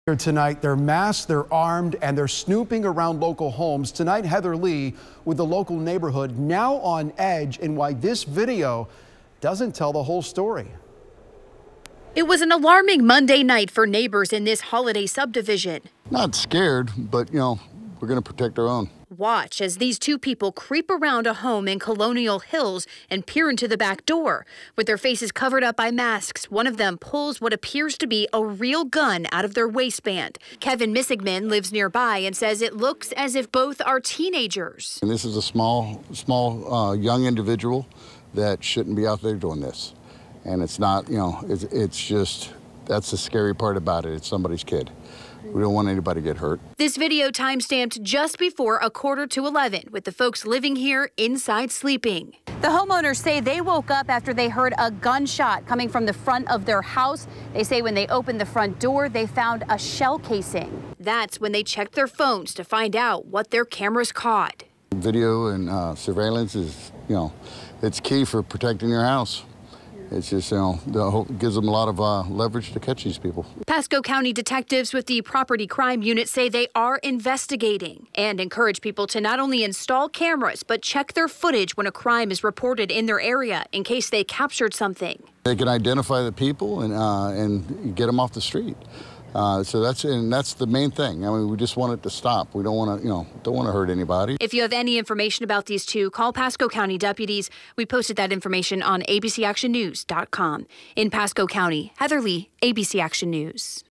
Ring camera captures two people in ski masks with a gun attempting to break into Holiday home
A Ring camera captured masked and armed people around Pasco County homes.